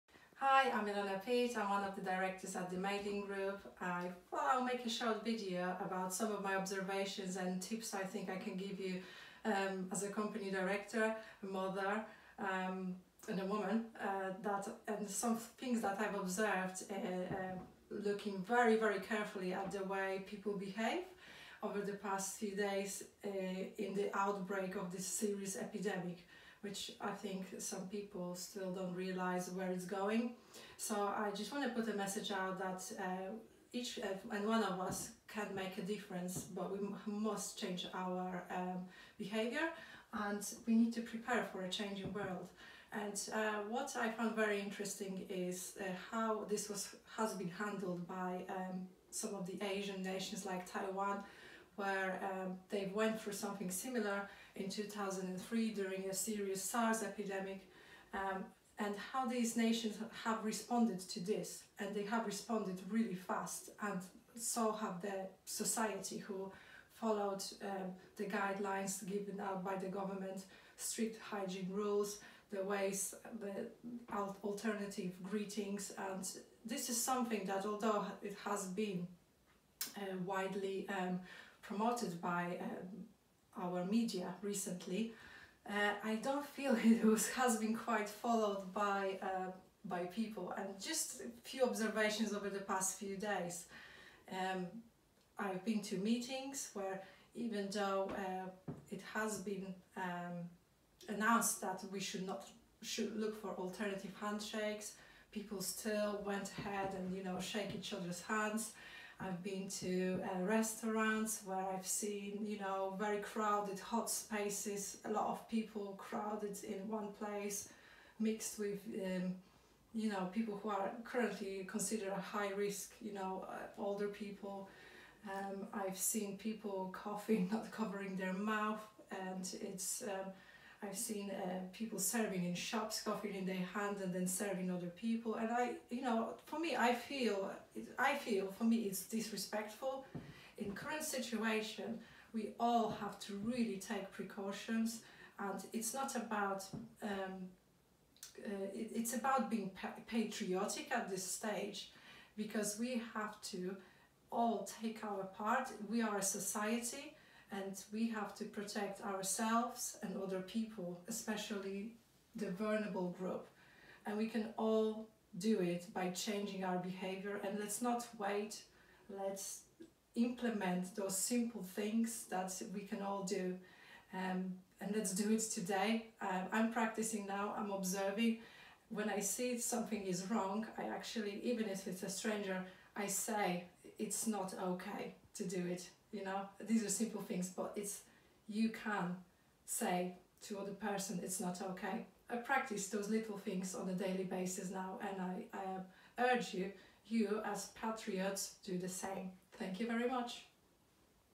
Change your behaviour today and protect our society
Ilona Pitt talking about importance of changing our daily behaviours in order to protect our society in the era of Covid-19